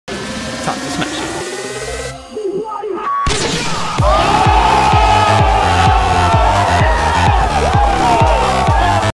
TIME TO SMASH IT / Miniminter's amazing crossbar
By Simon / Miniminter. I just edited it. Would be awesome if Simon himself saw this.